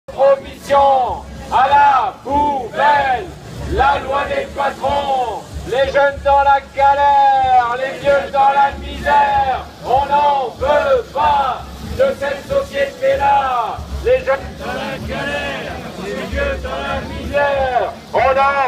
Manifestation contre la loi Travail à Travail : « De cette société, on en veut pas ! »
Dans les rangs des manifestants, les opposants à la loi Travail reprennent le slogan : « Les jeunes dans la galère, les vieux dans la misère, on veut pas de cette société là ! »